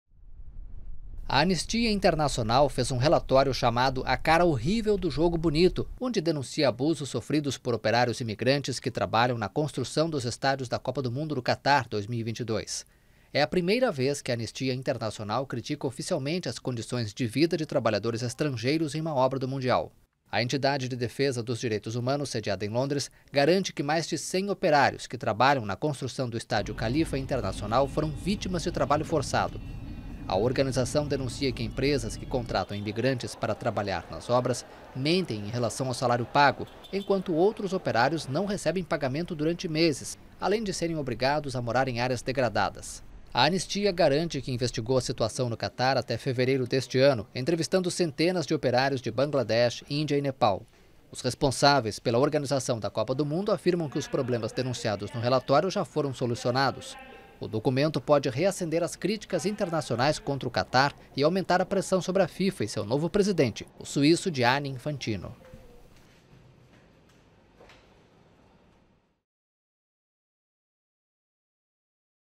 Anistia denuncia abusos de operários da Copa de 2022
Mais de 100 operários imigrantes que trabalham na construção dos estádios da Copa do Mundo do Catar-2022 sofreram abusos flagrantes e sistemáticos, incluindo trabalho forçado, afirma a Anistia Internacional em relatório que será publicado nesta quinta-feira.